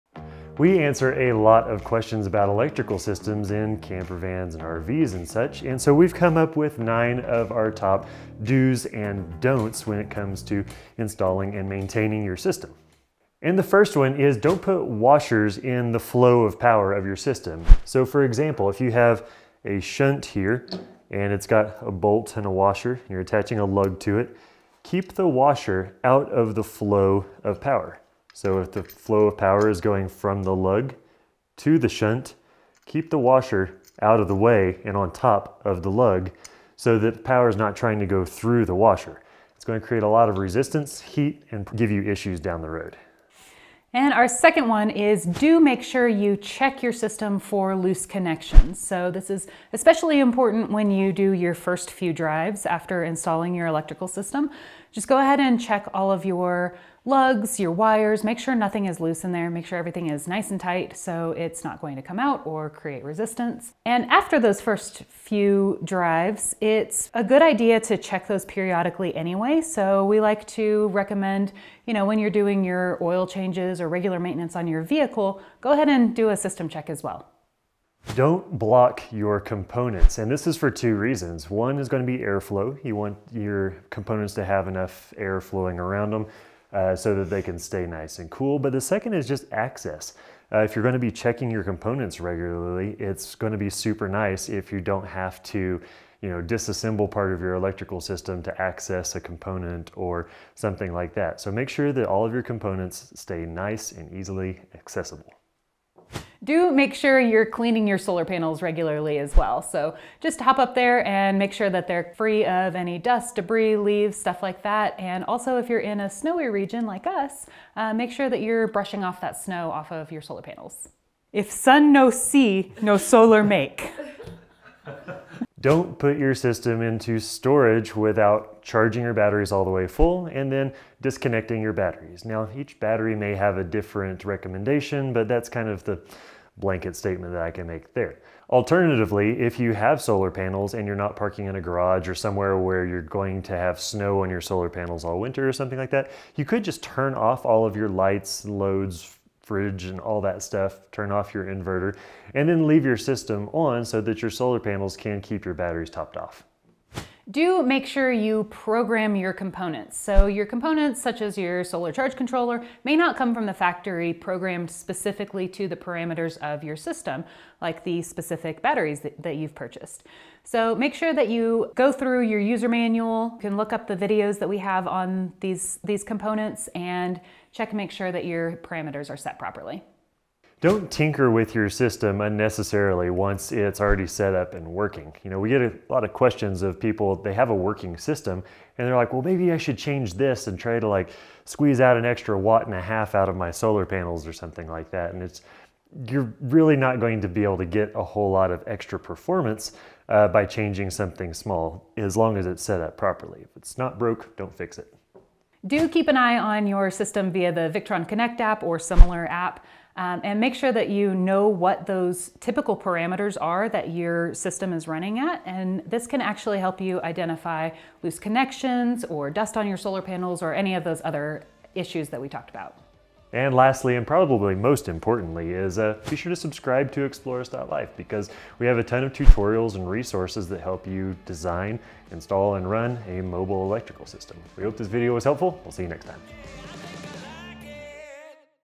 8 DIY CAMPER ELECTRICAL SYSTEM DO'S AND DON'TS
Here are 8 quick tips to make maintaining and operating your DIY Camper Electrical system easier.